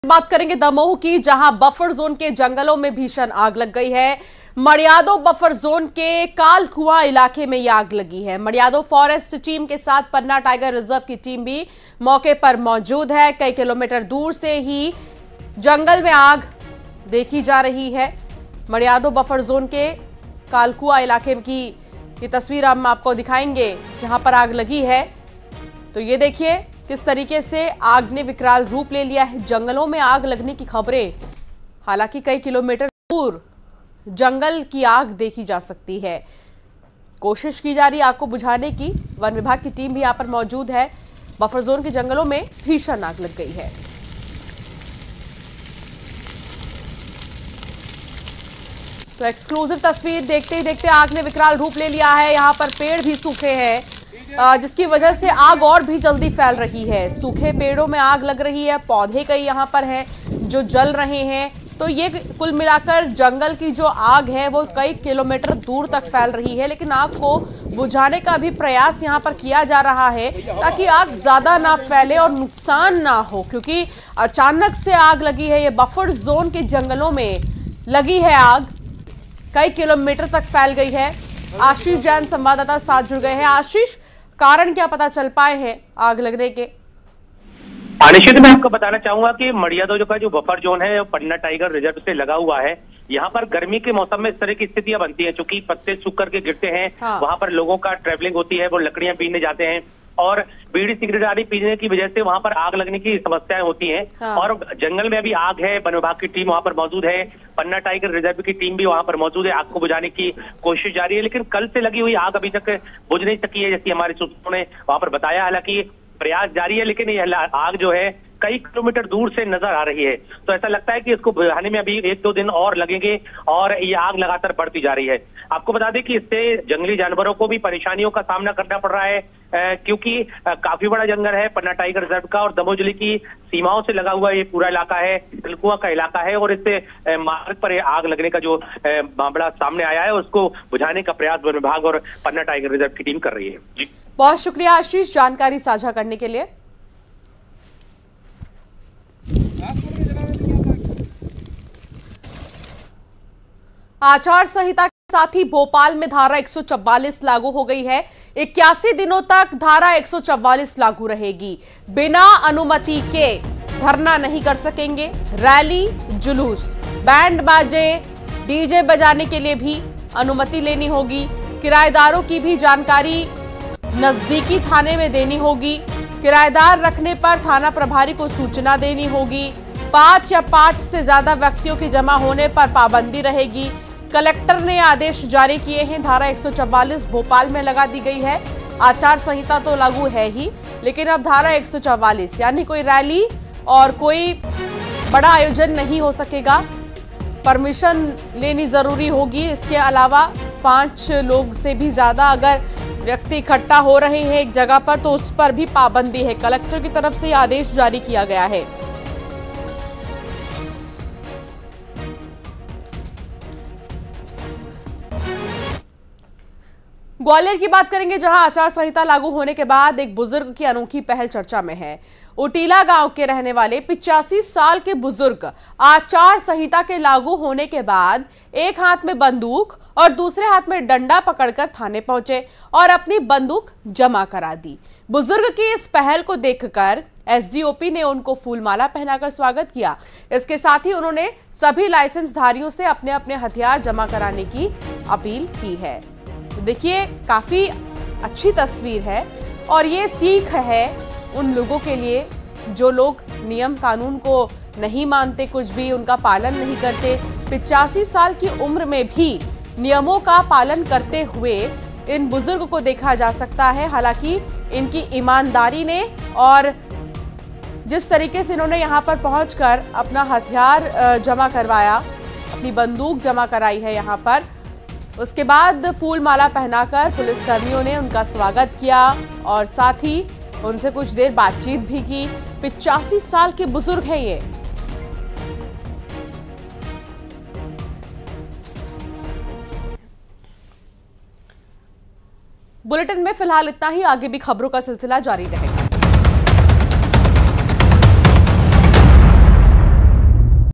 Breaking News : Damoh में मडियादो बफर जोन के कालकुआं इलाके में भीषण आग | MP News | Latest News |Fire
Breaking News : Damoh से बड़ी खबर है. बफर जोन के जंगलों में भीषण आग लग गई. मडियादो बफर जोन के कालकुआं इलाके में भीषण आग लगी है.